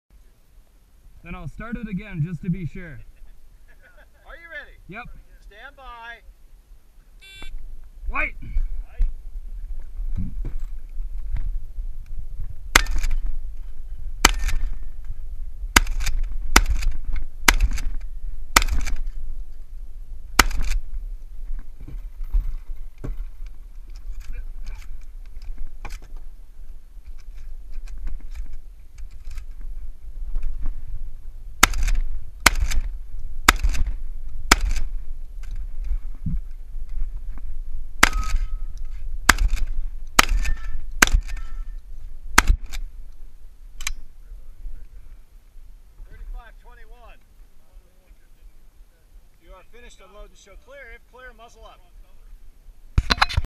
Pikes Peak Shotgun Challenge stage 8
This was 14 rounds of birdshot, and the targets that were shoot/no-shoot varied with each shooter. The RO would flip a coin outside of the shooter's sight, then lay it on top of the barrel behind the wobble platform, and finally cover it with a clay.
The shooter had to uncover the coin to reveal the color of the clay to shoot (hence me yelling "WHITE!"), and shoot only those assigned clays. All steel were valid targets, but they would throw either a shoot or a no-shoot.
Complicating matters was the wobble platform, and it was also considered a procedural per shot fired to shoot with any part of your body resting against the rails. The RO had to see whether any of your weight was bearing down on the platform.
Lots of people got ass faults.